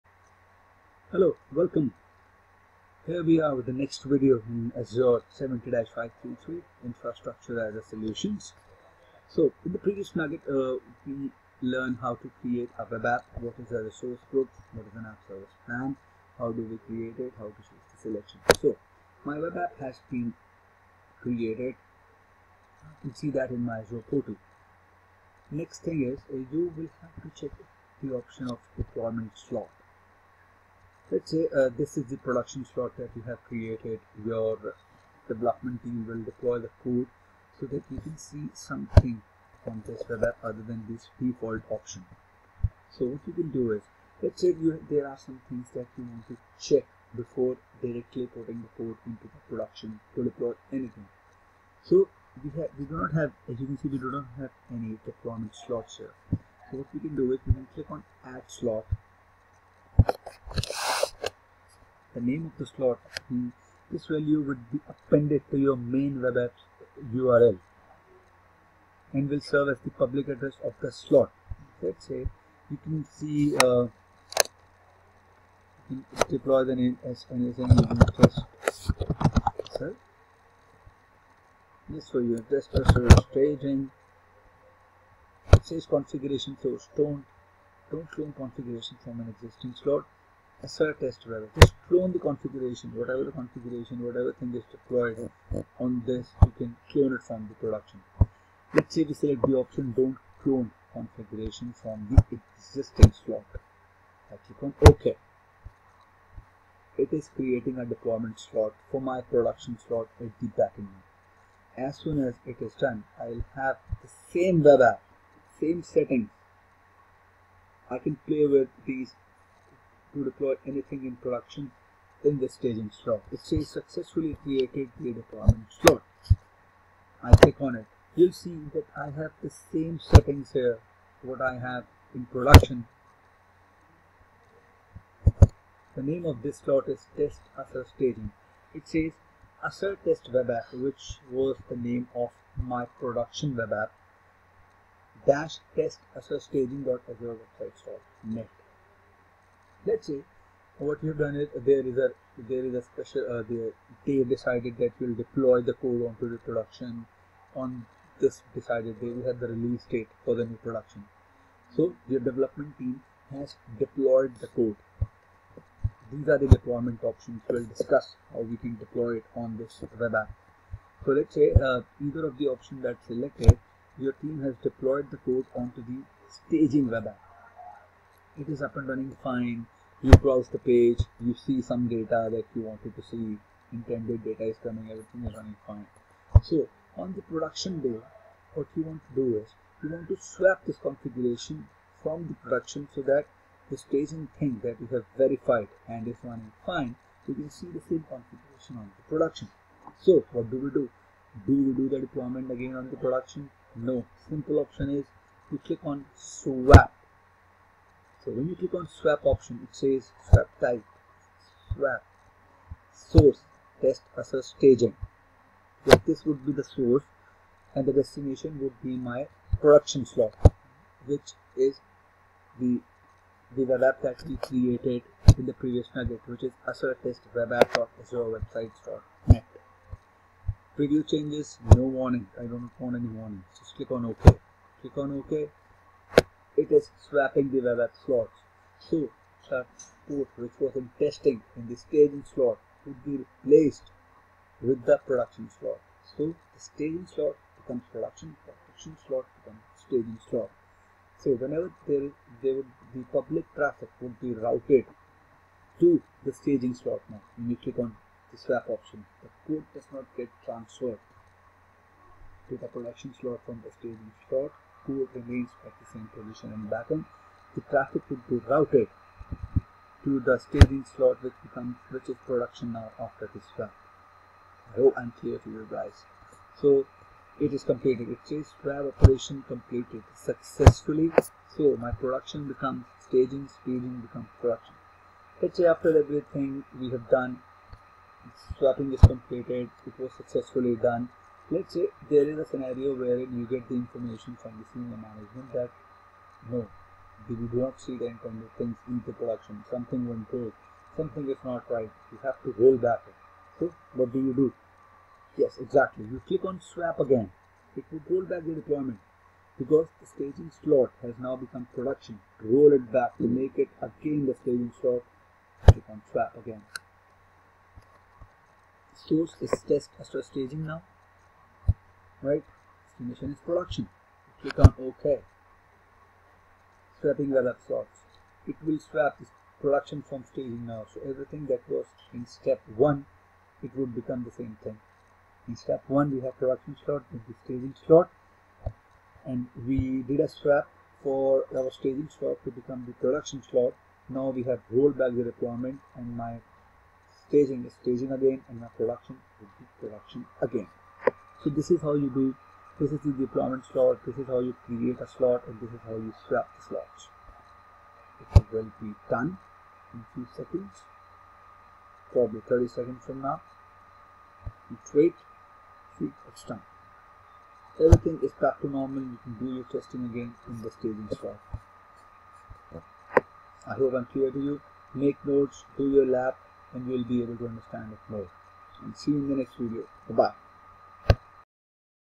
Video 4 - What is a Deployment Slot in Azure Web App
Microsoft Azure Training (70-533)
In this video, we will learn about the deployment slot in Azure Web App.